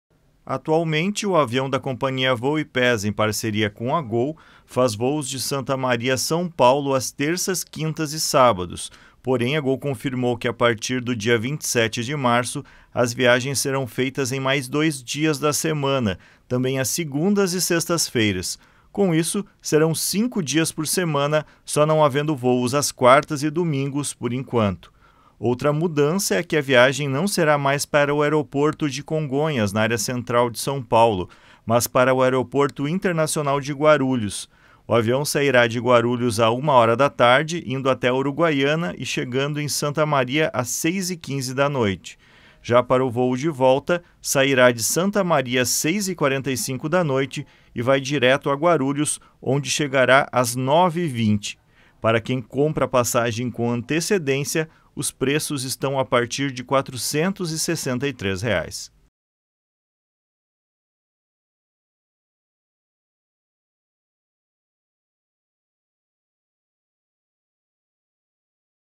Gol passará a voar 5 vezes por semana de Santa Maria a São Paulo
Veja as principais mudanças e para qual aeroporto será.

Que tal acompanhar o Diário e ficar por dentro das últimas notícias?